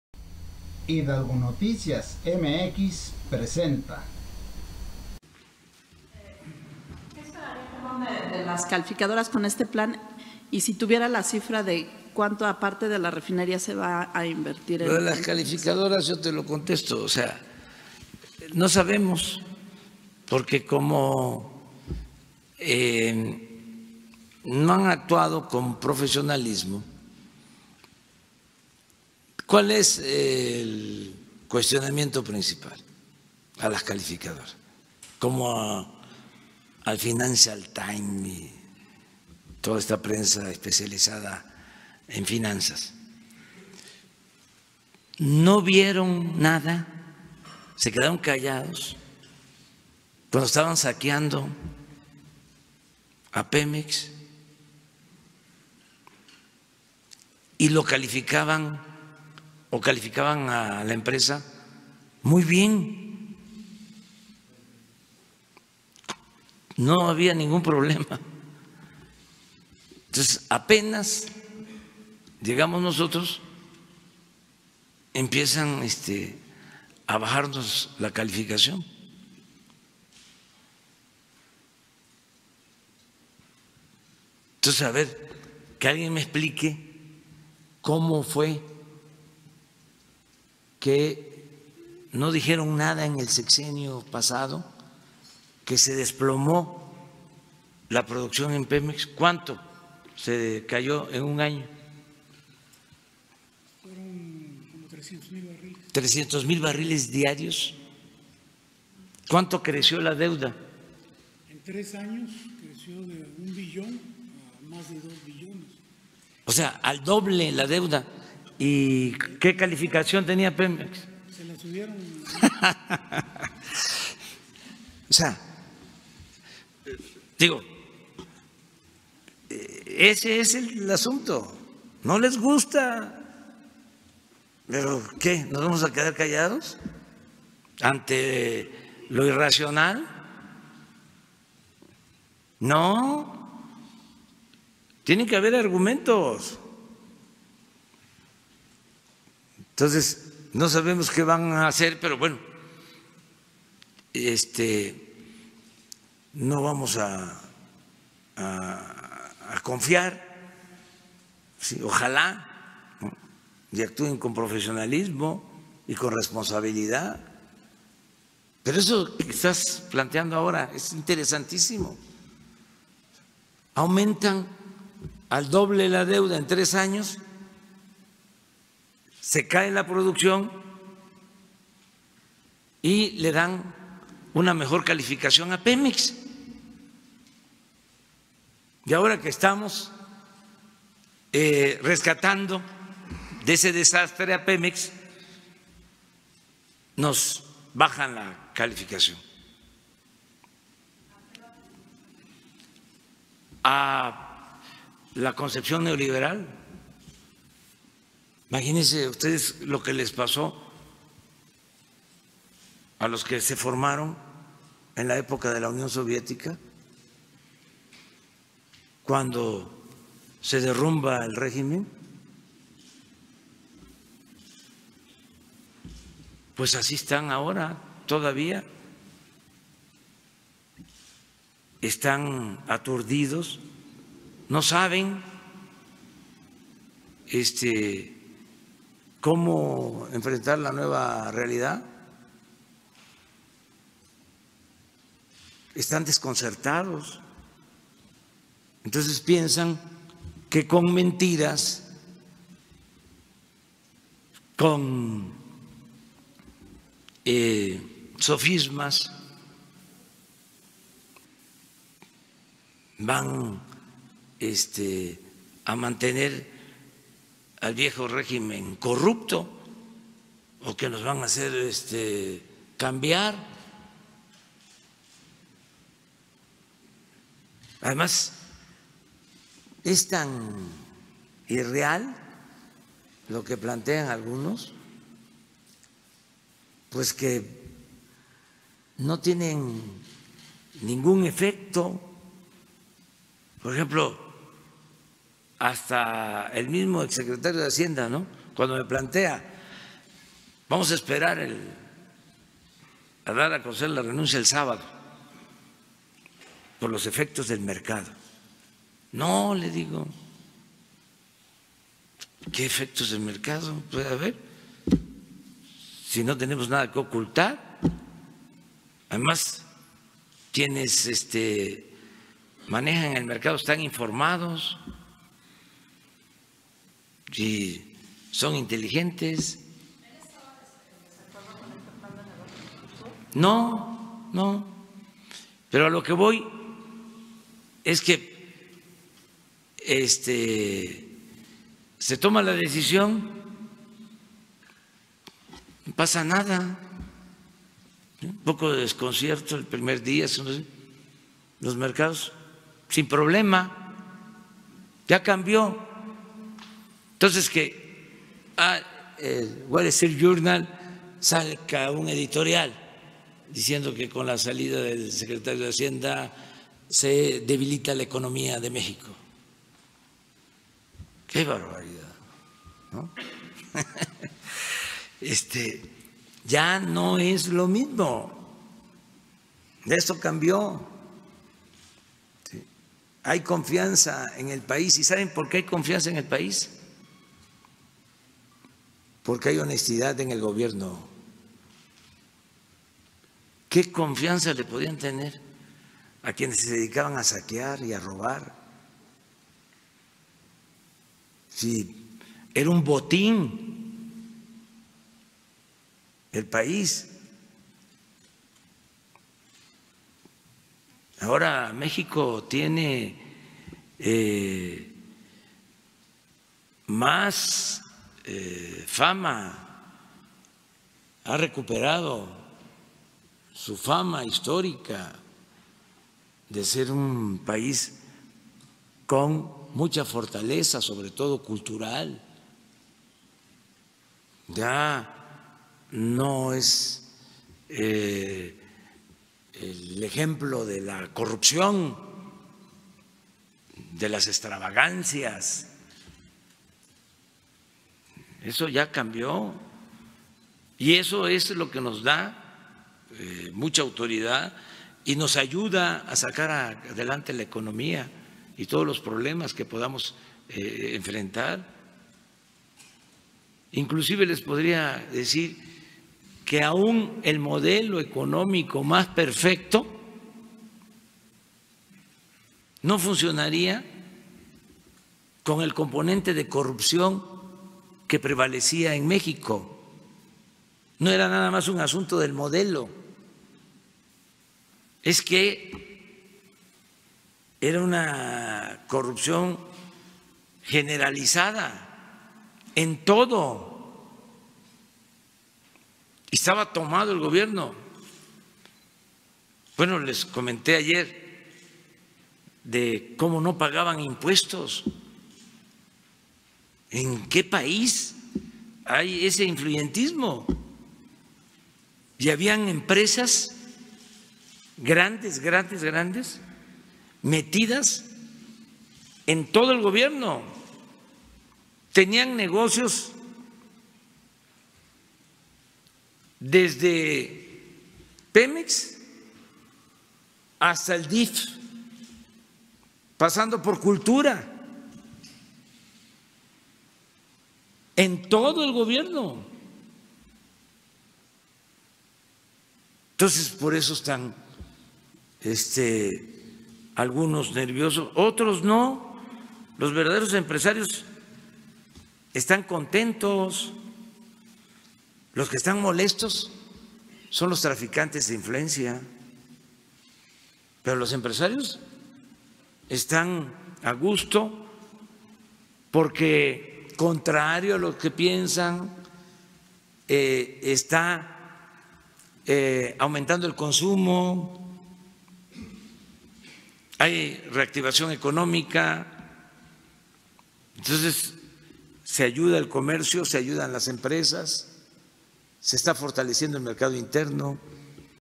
AMLO: No vamos a confiar en las "calificadoras", porque ellas nos evalúan mal.
Pese a que se tiene un Plan de Negocios y se está rescatando a PEMEX, las calificadoras nos evalúan muy mal. ¿Y quién califica a las calificadoras?